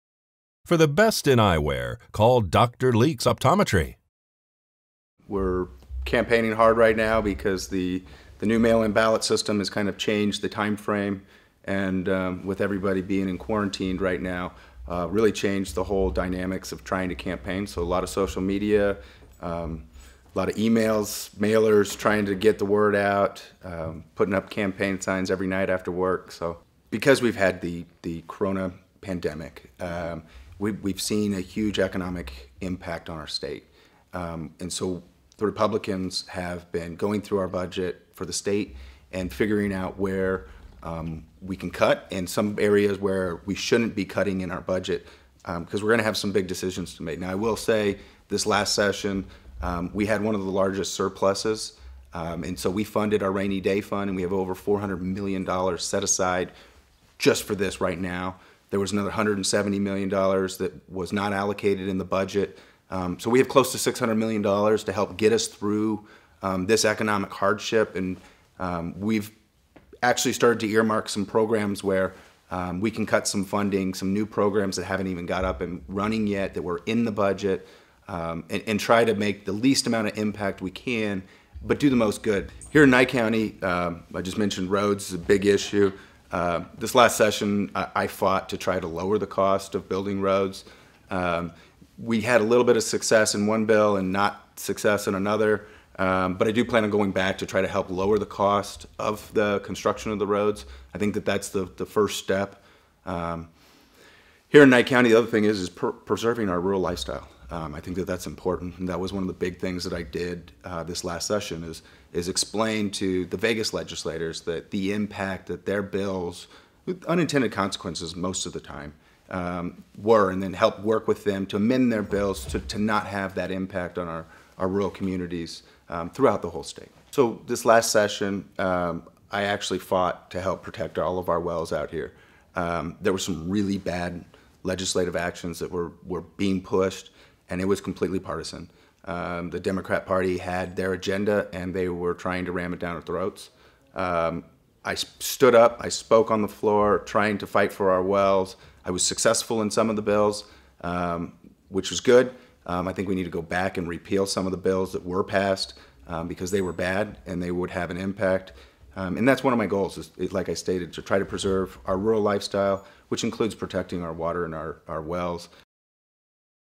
05-07-2020 Meet The Candidates: Greg Hafen, Candidate for Nevada Assembly District 36
Nevada Assemblyman Gregory Hafen has served the 36th district for two years.  And the Pahrump resident is hoping to serve another two years.  He's running for re-election in the June 9th primary.  And he says he looks forward to picking up where he left off.  To learn more about Gregory Hafen's campaign for re-election, you can visit his website -- Hafen4Nevada.com